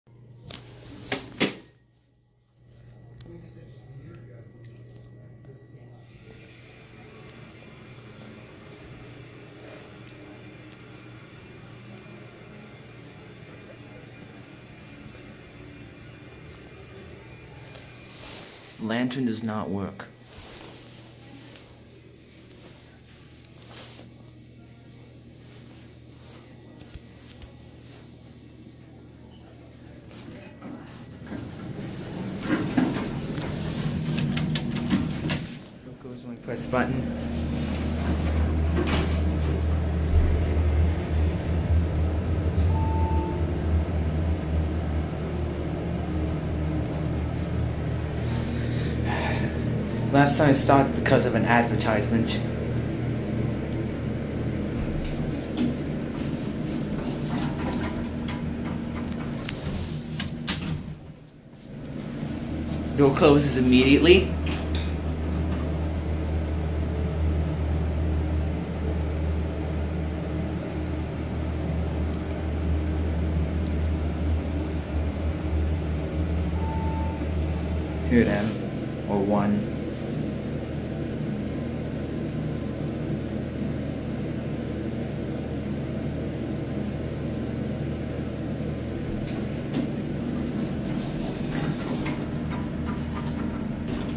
Another Older Take: 1978 Otis/GAL Hydraulic Elevator @ The Bon/Macy's, Alderwood Mall, Lynnwood WA
This was taken a couple days after the bumpers were removed.

Taken 18 February 2019

Installed 1978